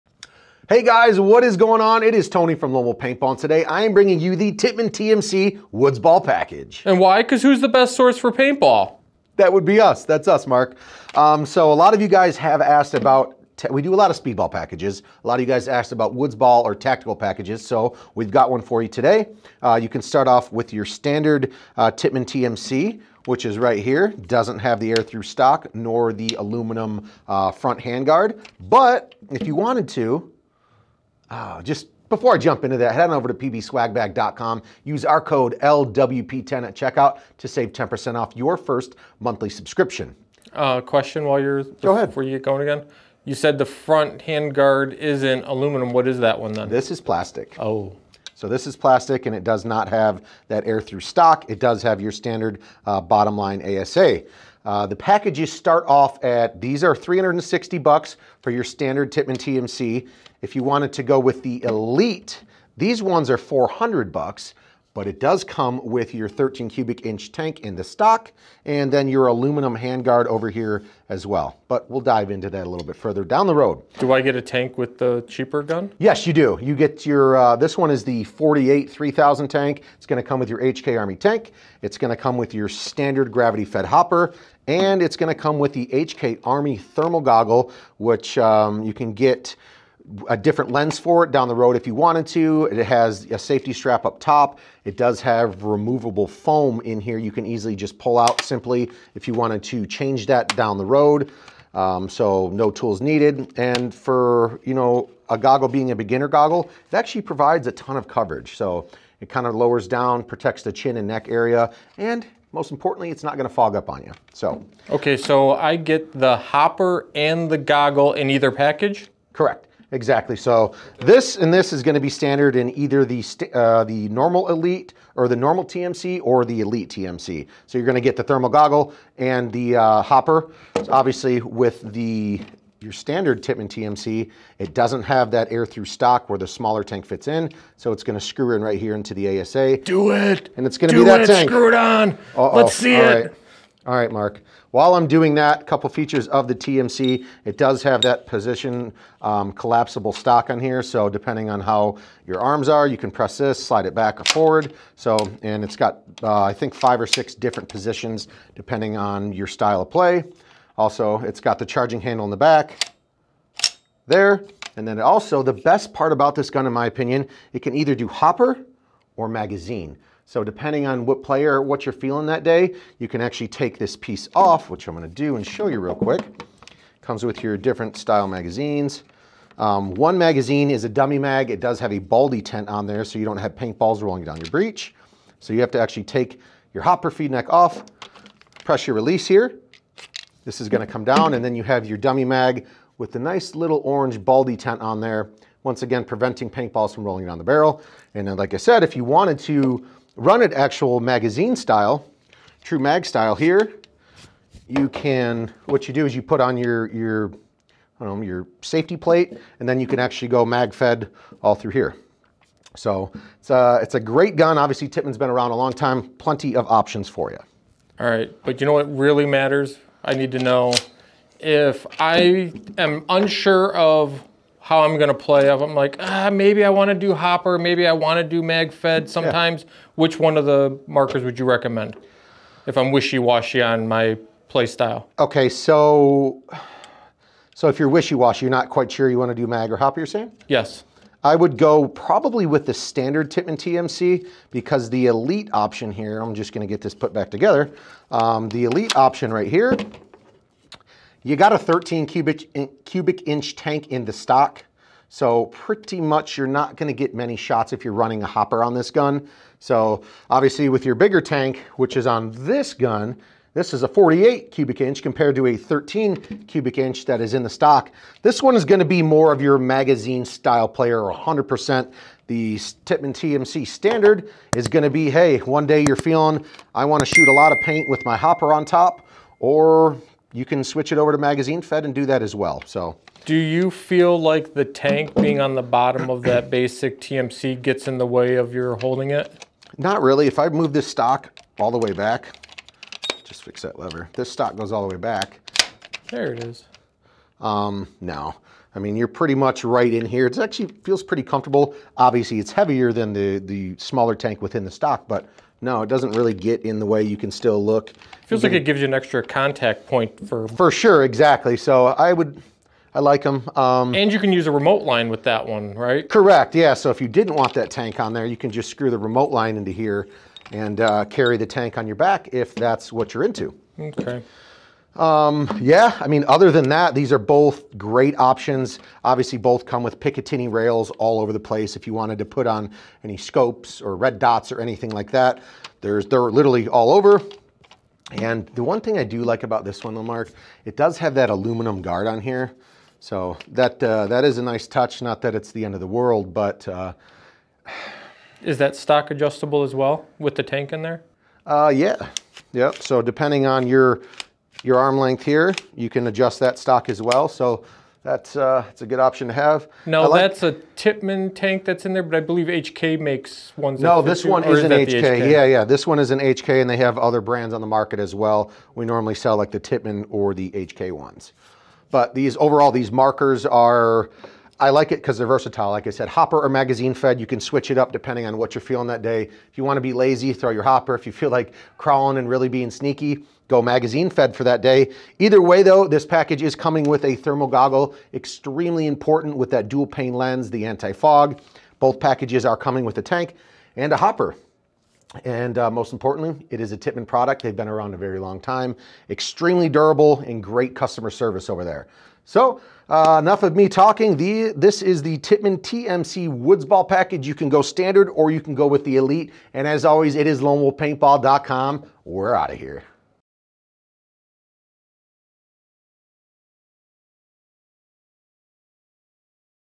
Tippmann TMC Beginner Woodsball Paintball Packages | Lone Wolf Paintball
Tippmann TMC  Beginner Woodsball Paintball Packages
Tony reveals two Tippmann TMC woodsball packages that are now available on our website. Available in with the basic TMC or the TMC Elite.
Order the Tippmann TMC Package from the best source for paintball

00:00 Intro
00:19 Tippmann TMC Beginner Package
00:32 PB Swag Bag
00:56 Package Prices
01:05 Package Options
03:37 Which package if unsure of play style?
04:42 Does the tank get in the way of the Tippmann TMC?
05:48 Is the stock adjustable on the TMC Elite?
06:30 Closing